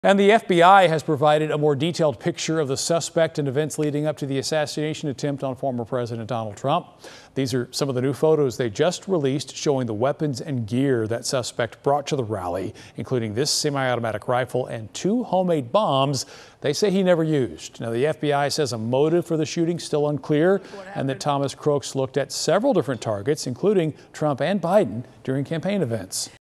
FBI releases new information on weapons carried by attempted Trump assassin
He also carried two bombs that he did not use.